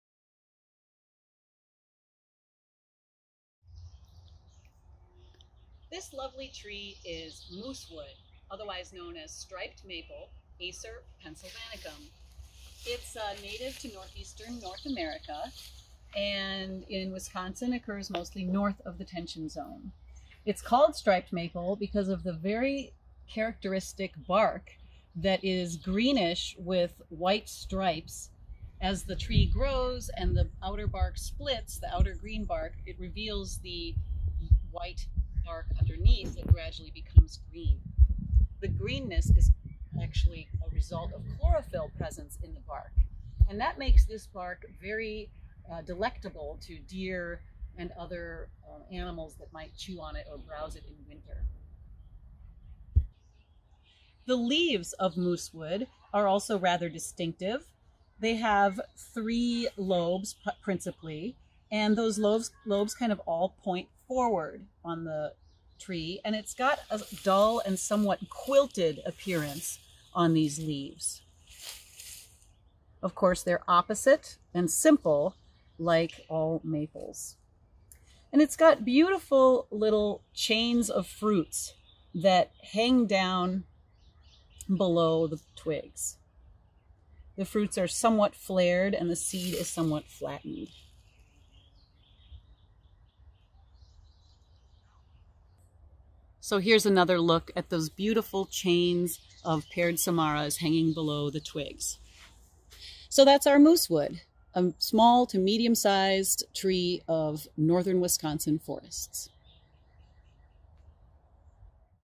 Acer pensylvanicum - moosewood
Dr. Catherine Woodward explains the botanical characters for identifying moosewood, also known as striped maple.